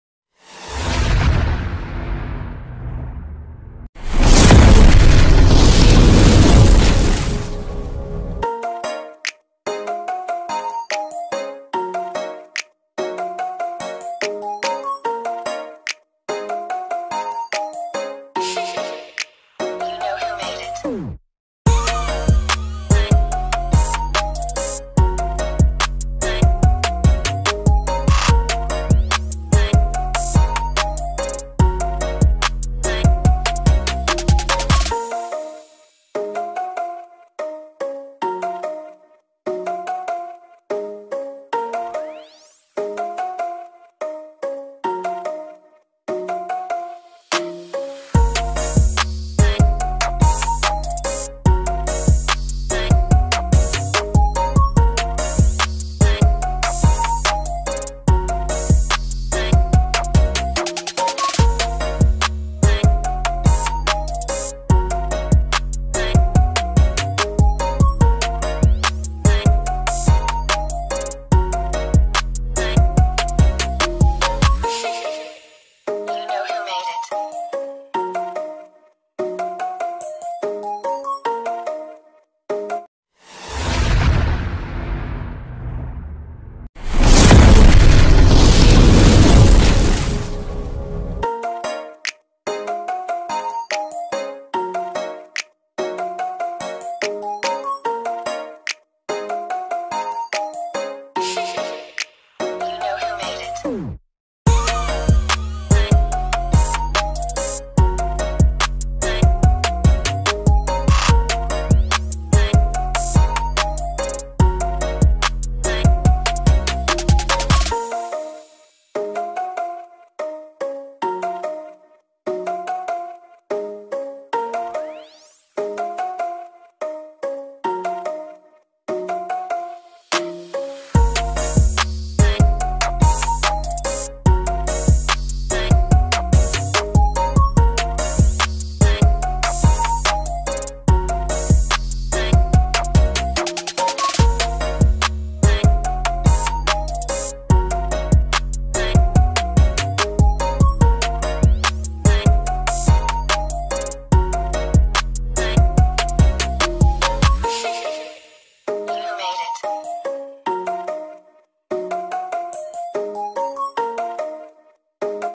HOW TO MAKE PLYWOOD RED FORT || INDEPENDENCE DAY CELEBRATION
15th August INDEPENDENCE DAY CELEBRATION 🥳🎉
Material used

1. waterproof colour (Distemper)
2. brush
3. pencil
4. Plywood